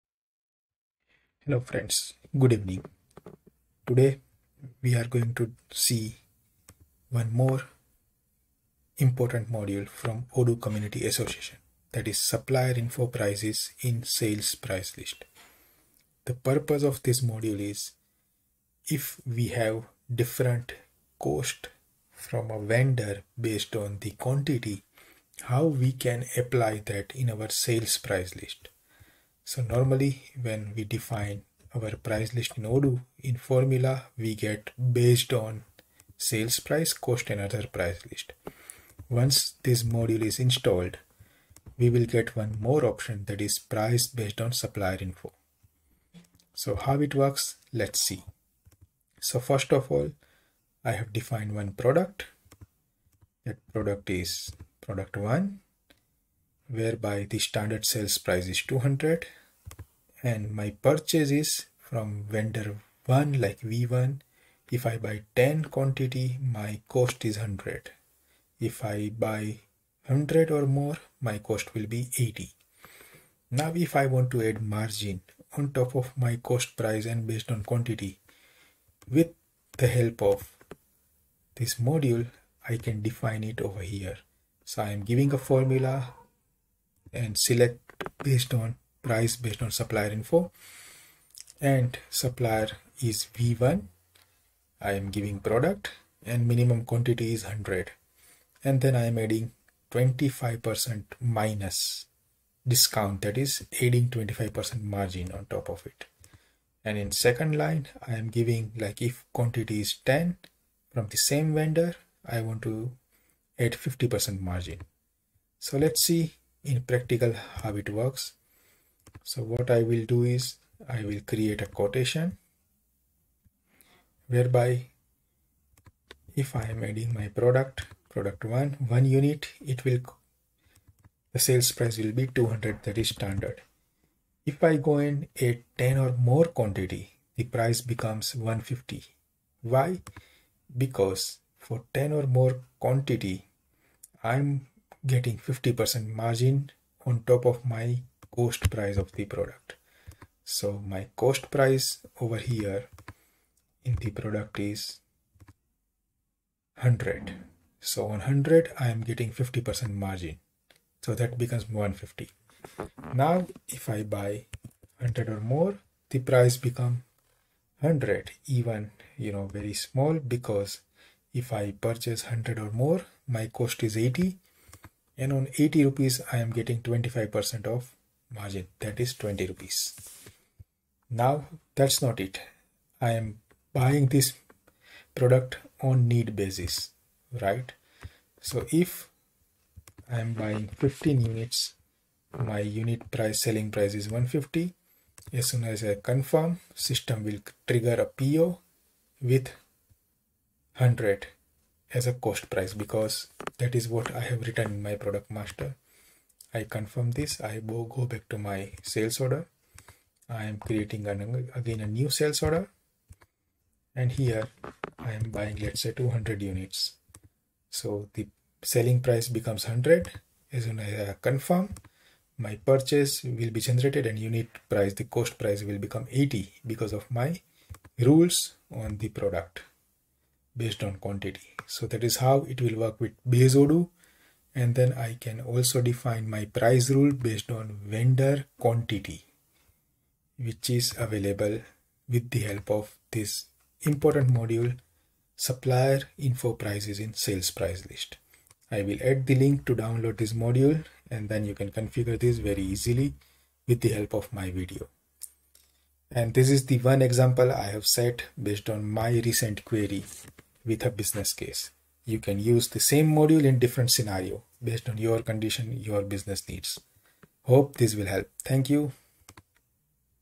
How to offer best sale price based on Supplier cost in Odoo sales pricelist using FREE OCA App
The Supplier info prices in sales pricelists module brings feature in Odoo Sales Pricelists to offers several benefits, particularly for businesses that calculate sales prices relative to their supplier costs based on quantity.

Here are some of the advantages:
1. Dynamic Pricing Based on Supplier Costs
Sales prices automatically adjust according to changes in supplier costs based on quantity. This ensures your pricing remains competitive and reflects current procurement expenses without manual updates.

2. Improved Profit Margin Management
You can define margins (percentage or fixed amounts) over supplier costs to maintain consistent profit levels. This reduces the risk of under pricing or overpricing your products.

3. Transparency in Pricing
Using a supplier-cost-based method makes it easier to justify prices to customers and stakeholders by demonstrating that sales prices are tied to procurement costs.

4. Efficiency for Wholesale and Resale Businesses
For businesses with high procurement and sales volumes, this feature ensures that pricing remains synchronised with supplier agreements, making bulk operations more manageable.